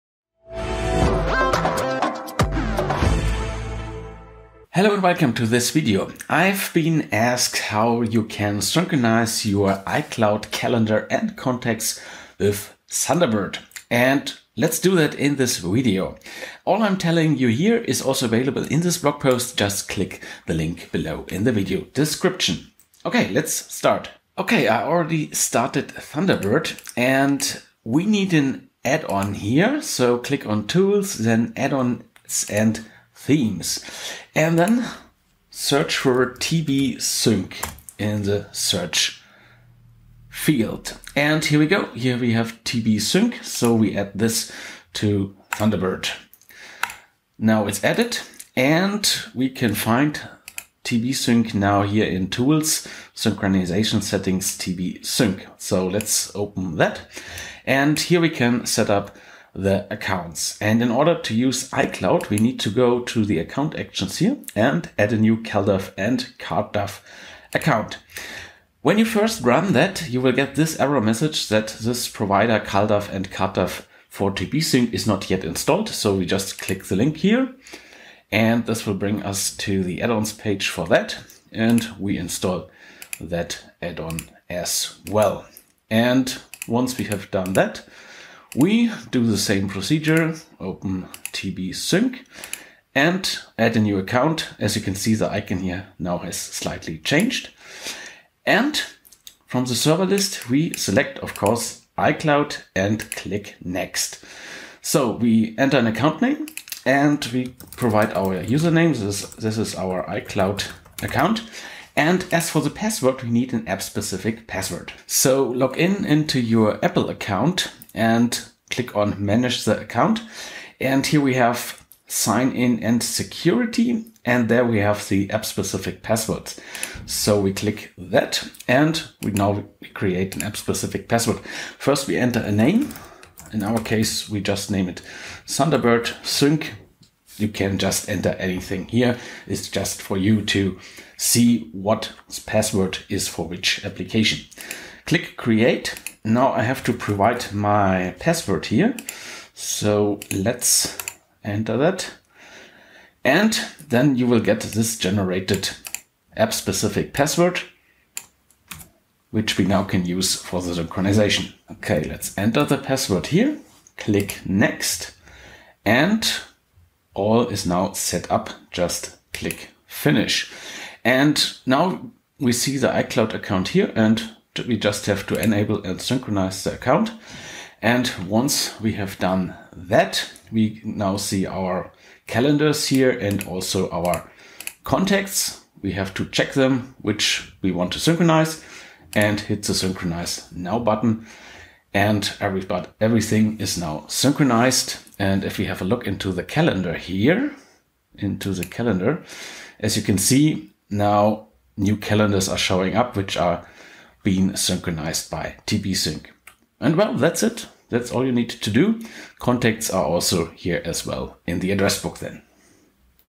Synchronize iCloud calendar and contacts with Thunderbird
Synchronize iCloud calendar and contacts with TbSync in Thunderbird.

0:00 Start and introduction
0:24 Install TbSync, start configuration
1:54 App specific passwords
2:41 Finish configuration and synchronize data